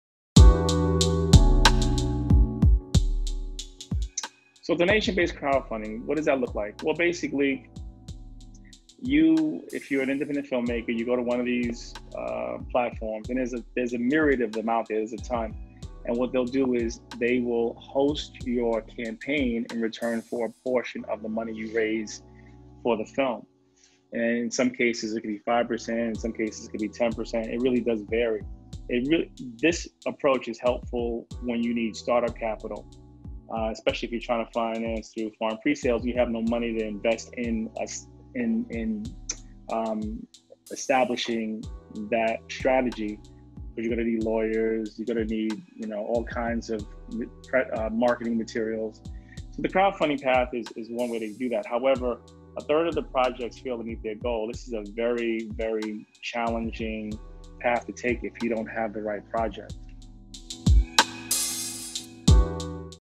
Donation-Based Crowdfunding [Clip 5 of 13]
Despite the normal volatility in the film industry, it remains as robust as ever and is projected to grow to $47.9 billion dollars by 2022, slightly outpacing the U.S. economy. Prior to the latest wave of media consolidation, seven major studios controlled four-fifths of the film industry’s revenue in 2017. Superhero films and other costly genres represent a large portion of that revenue and have dominated the marketplace. This over-indexing in the production of genre films is a result of the major studios trying to generate massive audiences by producing tent pole pictures. The combined industry box office revenue in 2018 was almost 3.3 billion, largely due to the success of genre films. However, these types of revenues don’t cultivate much of an appetite in the industry for funding arthouse films. Nor does this trend of massive budgets bode well for the inevitability bursting the spending bubble or a mega picture flop.

The increased fragmentation of viewing audiences, the shrinking theatrical windows, and the exploitation of the indie film market by streaming services like Netflix, has shifted the economics of the film business. From the arthouse moviemaker to the major studio producer or film financier/venture capitalist, everyone is looking for new diversified investors to help mitigate their risk and stabilize their investments through strategic co-financing models. That’s why many in the entertainment business see the emergence of crowdfunding and cryptocurrency as the answer to some of these film financing dilemmas. Through changes in equity crowdfunding legislation combined with new technological advances like blockchain, producers may be able to effectively hedge their costly investments, while generating a more expansive investor pool to fund the dearth of arthouse pictures.